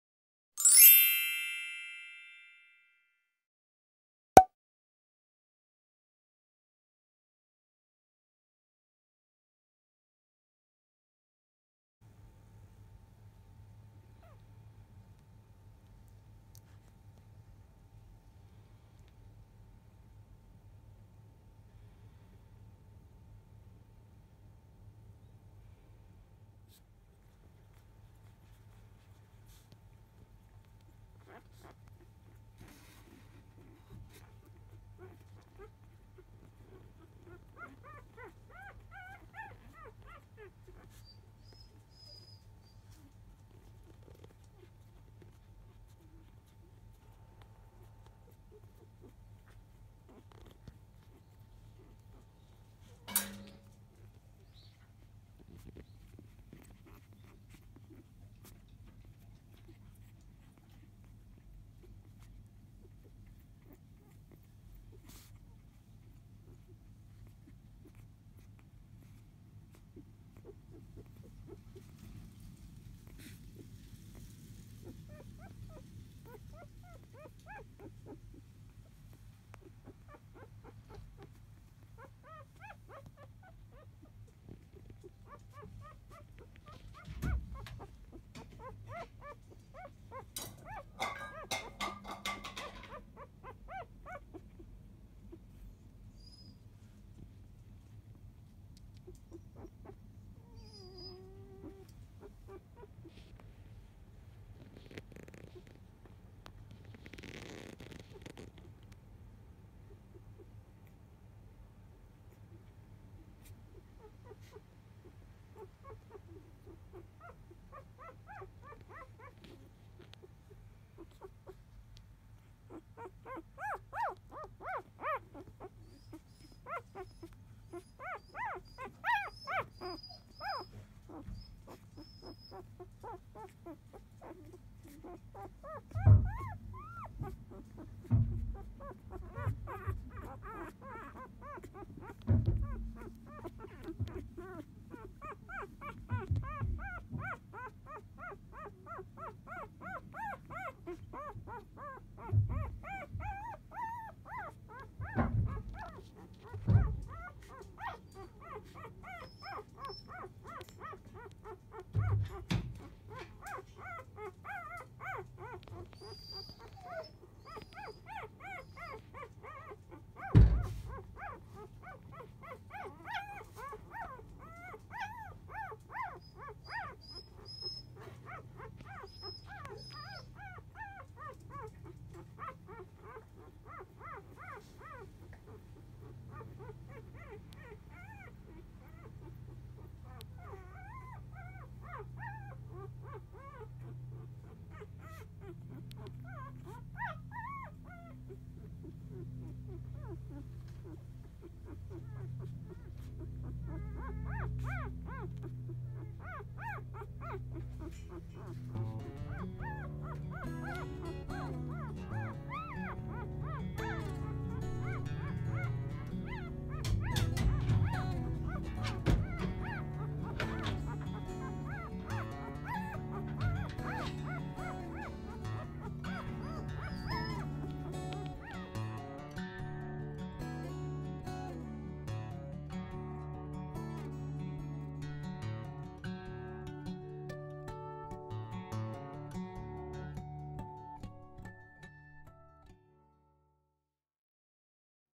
ラブラドールレトリーバーの子犬・生後4日目・こいぬのひるね♪
2017年JFAグランドチャンピオンのbiscuitが出産♪